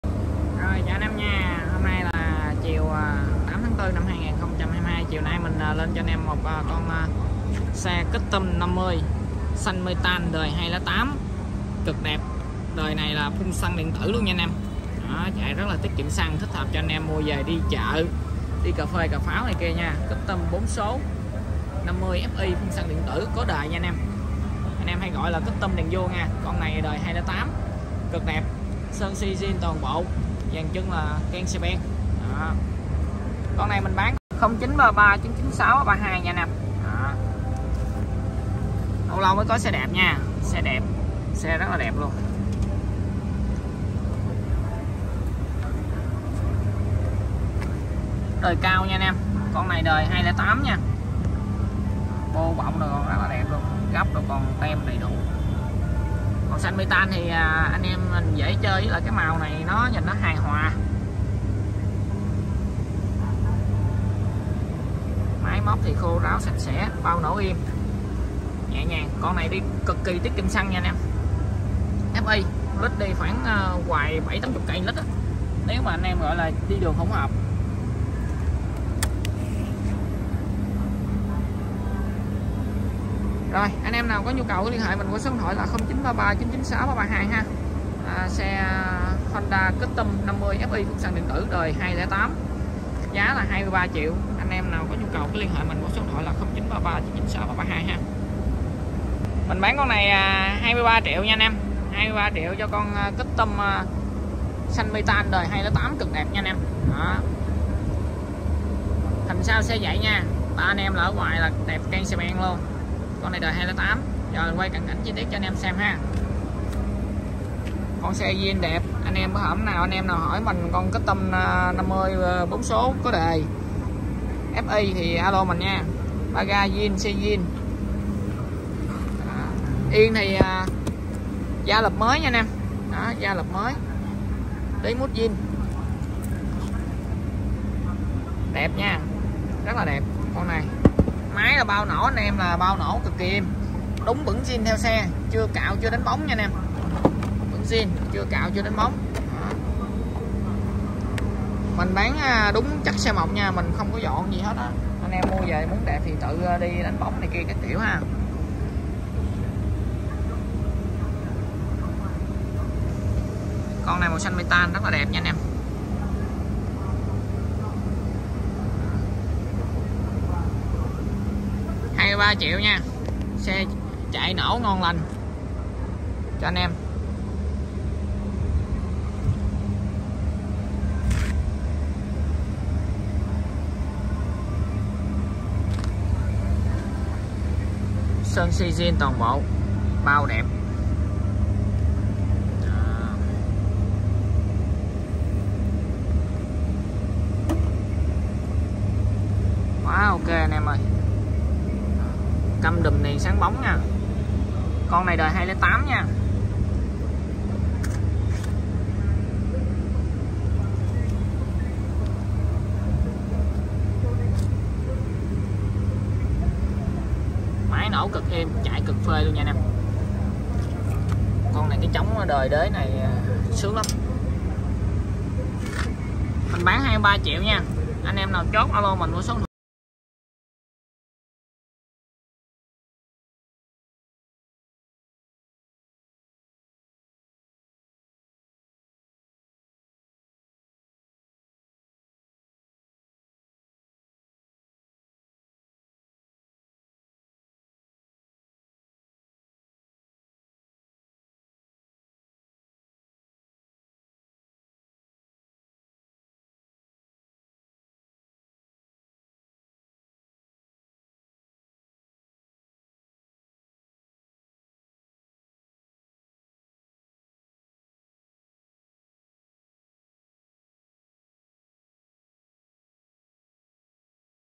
🆘 ĐÃ BÁN ➡ Xe CUSTOM 50 Fi Đời 2007 Đèn Vuông Xanh Metan Phun Xăng Điện Tử Cực Đẹp ✅Giá : 23 Triệu
🆘 8/4/2022
➡ Xe CUSTOM 50 Fi Đời 2007 Đèn Vuông Xanh Metan Phun Xăng Điện Tử Cực Đẹp
✅ Giá : 23 Triệu / Xe + Bửng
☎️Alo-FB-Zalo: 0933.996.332

🆘 8/4/2022

❗Sơn Xi Zin ko dặm ko tút

❗Máy móc nguyên Zin - Êm ái

❗Xe nổ máy chạy bình thường

+ Agribank, CN Bình Tân, HCM
+ Đông Á Bank, CN Trường Chinh , HCM
+ Sacombank , CN Hốc Môn, HCM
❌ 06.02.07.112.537